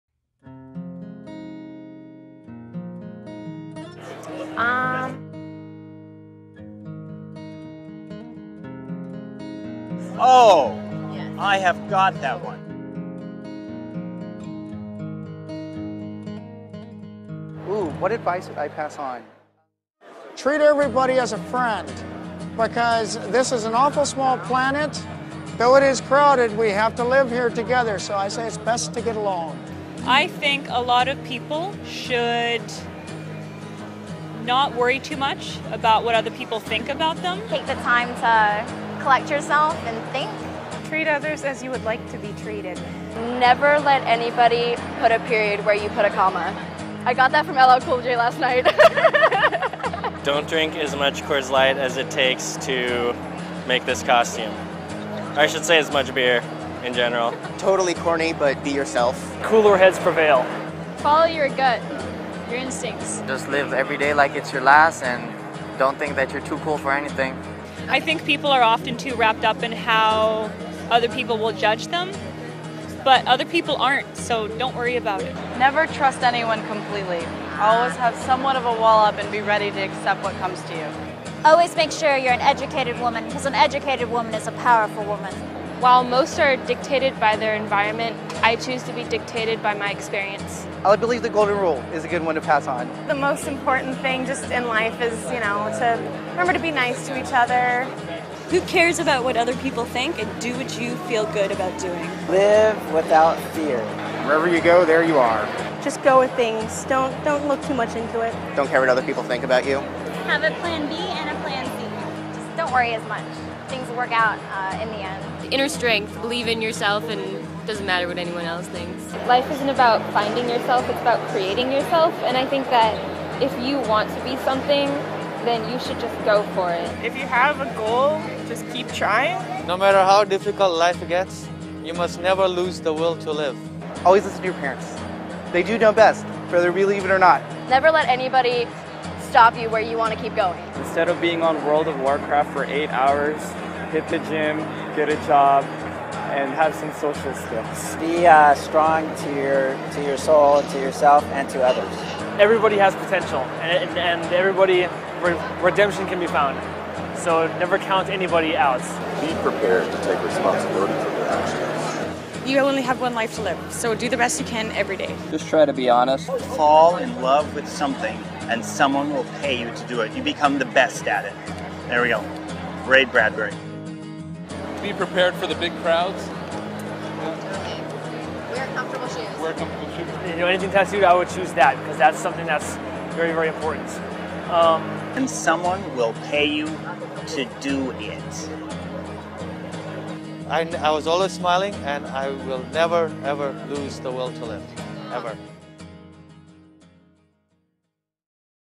Episode 124: Cosplayers on Life
FROM THE MOUTHS OF BABES compilation video number 24, featuring cosplayers.

In this video:
Alicia and Ashley
Anthony
Christopher
Christy
Eric
Humbert and Ariel
Jaeson
Jared
Jessica
Jessica and Brigitte
JR and "Chuck Norris"
Kevin, Jeff, and Amy
Kimkashi
Kyle
Michelle
"Poison Ivy" and "Batman"
Rizwan
Russell
Samantha
Sara and Alys
Savannah
Sean
Sonya

New interviews added Mon/Wed/Fri.